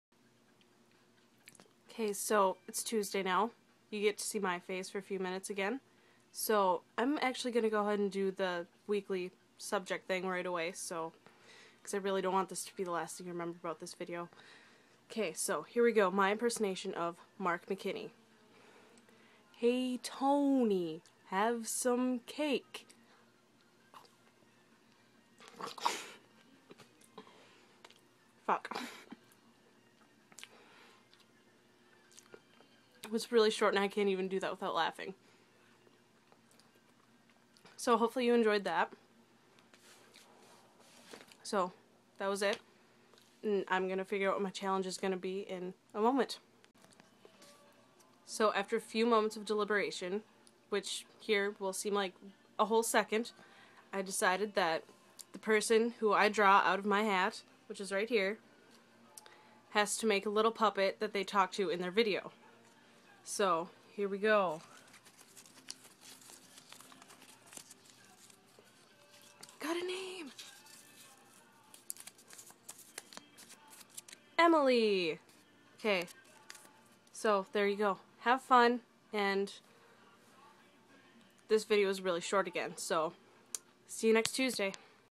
Rainbow Cake, Mark McKinney and a Mohawk hat
My very short impersonation of Mark that I could hardly get through. That cake was a blast to make though!